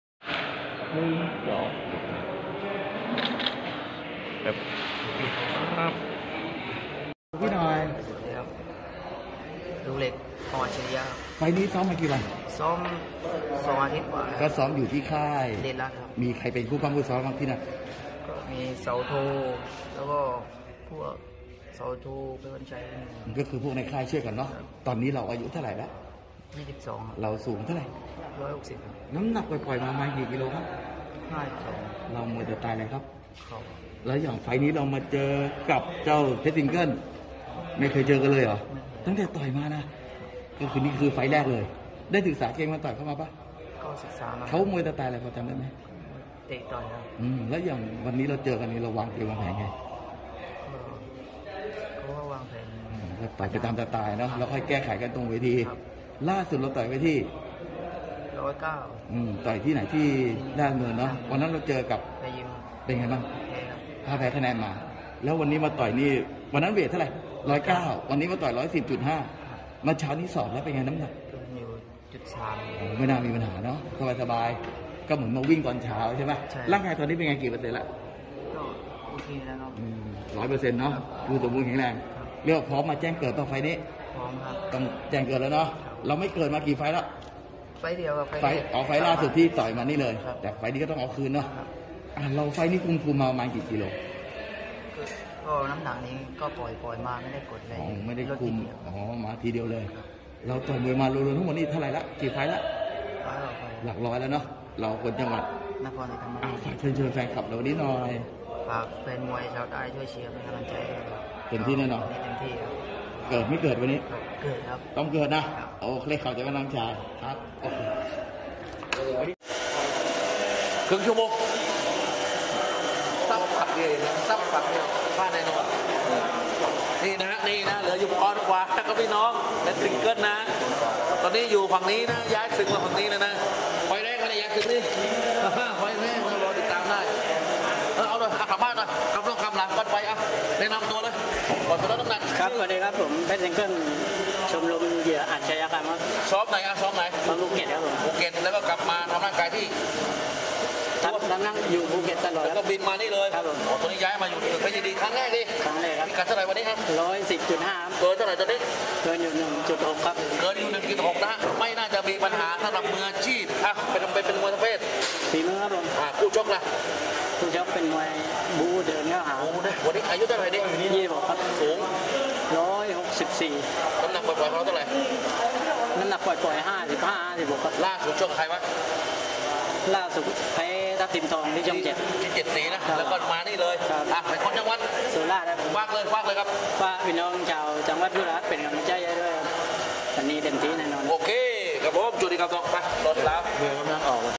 สัมภาษณ์ กระดูกเหล็ก VS เพชรซิงเกิ้ล ศึกเพชรยินดี เวทีมวยราชดำเนิน ทรู24 สด 18.00 น.
ติดขอบตาชั่ง|สัมภาษณ์ กระดูกเหล็ก VS เพชรซิงเกิ้ล ศึกเพชรยินดี เวทีมวยราชดำเนิน ทรู24+เพจมวยเด็ด789 สด 18.00 น.เป็นต้นไป

09/06/2565